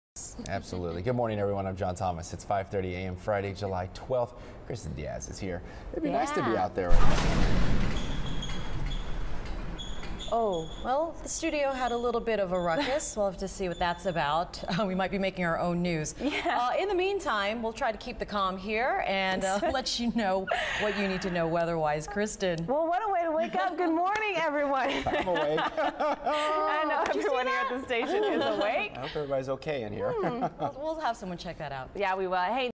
Corpus Christi home explosion reaction KIII live.
Corpus Christi explosion reaction KIII news anchors on live television.  John Thomas, Kristin Diaz, Jane Monreal